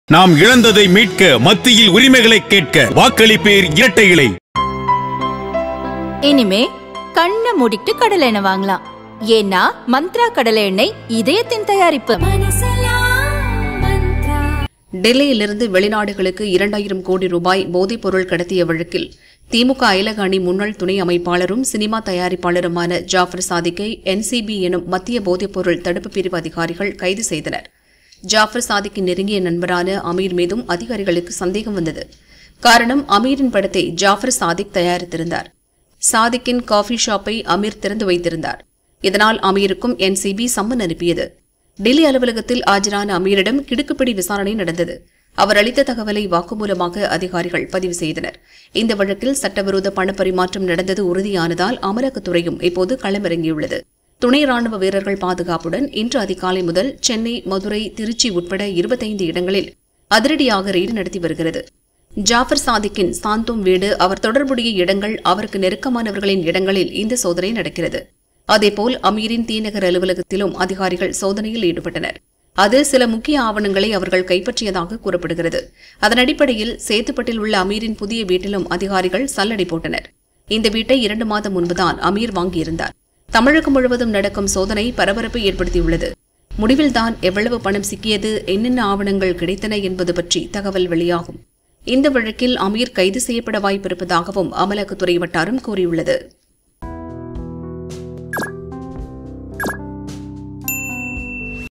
அமீர் ஆபீசில் ED எடுத்த முக்கிய எவிடன்ஸ்-அடுத்து கைது? | ED Raid | Director Ameer
அமீர் ஆபீசில் ED எடுத்த முக்கிய எவிடன்ஸ்-அடுத்து கைது? | ED Raid | Director Ameer | Jaffer Sadiq case#EDRaid